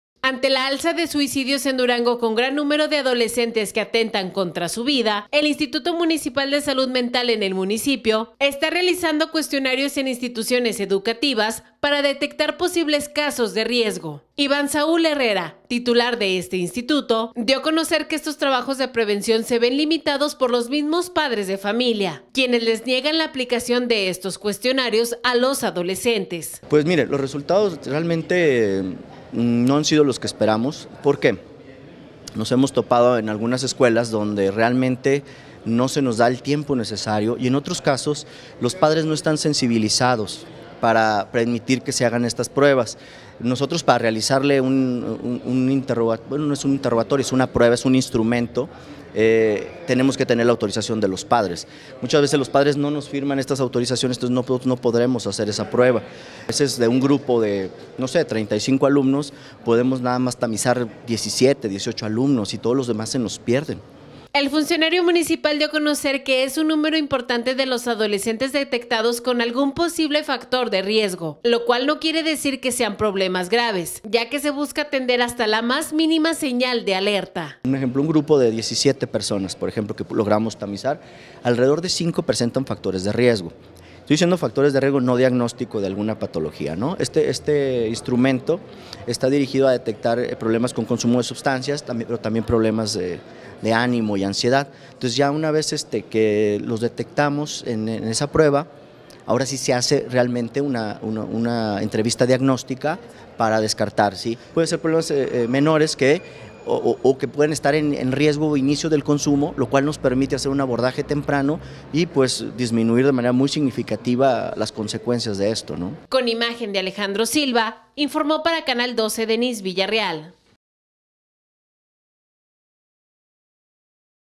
PADRES BLOQUEAN TRABAJO DE INSTITUTO | 23 MAYO 2018
El Instituto de Salud Mental en el municipio se ve limitado a realizar trabajos de detección de casos de riesgo en adolecentes, su titular Iván Saúl Herrera, dice que son los mismos padres quienes les niegan la aplicación de cuestionarios.